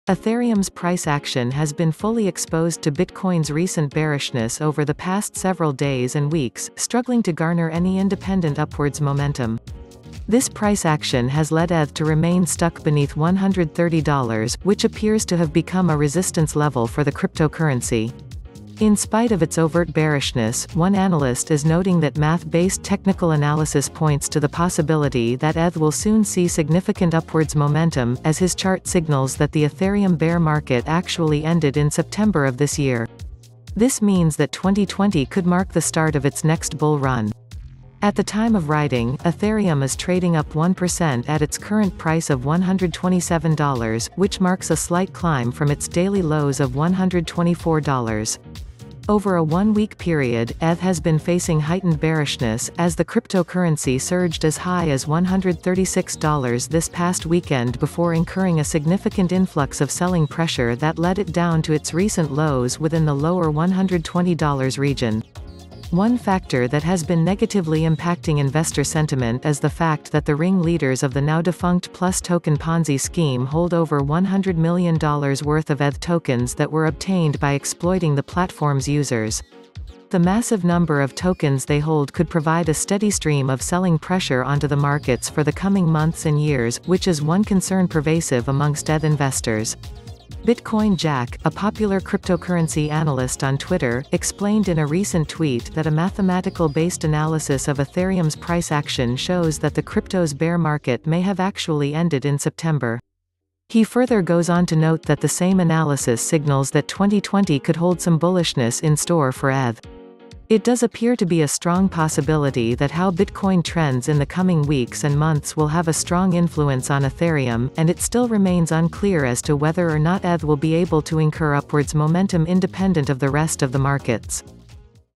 Math Shows That 2020 Could Be a Great Year for Ethereum Bulls; Here’s Why
Everyone should see it!

Ethereum’s price action has been fully exposed to Bitcoin’s recent bearishness over the past several days and weeks, struggling to garner any independent upwards momentum. This price action has led ETH to remain stuck beneath $130, which appears to have become a resistance level for the cryptocurrency. In spite of its overt bearishness, one analyst is noting that math-based technical analysis points to the possibility that ETH will soon see significant upwards momentum, as his chart signals that the Ethereum bear market actually ended in September of this year. This means that 2020 could mark the start of its next bull run. At the time of writing, Ethereum is trading up 1% at its current price of $127, which marks a slight climb from its daily lows of $124. Over a one-week period, ETH has been facing heightened bearishness, as the cryptocurrency surged as high as $136 this past weekend before incurring a significant influx of selling pressure that led it down to its recent lows within the lower-$120 region. One factor that has been negatively impacting investor sentiment is the fact that the ring leaders of the now defunct PlusToken Ponzi scheme hold over $100 million worth of ETH tokens that were obtained by exploiting the platform’s users. The massive number of tokens they hold could provide a steady stream of selling pressure onto the markets for the coming months and years, which is one concern pervasive amongst ETH investors. Bitcoin Jack, a popular cryptocurrency analyst on Twitter, explained in a recent tweet that a mathematical-based analysis of Ethereum’s price action shows that the crypto’s bear market may have actually ended in September. He further goes on to note that the same analysis signals that 2020 could hold some bullishness in store for ETH. It does appear to be a strong possibility that how Bitcoin trends in the coming weeks and months will have a strong influence on Ethereum, and it still remains unclear as to whether or not ETH will be able to incur upwards momentum independent of the rest of the markets.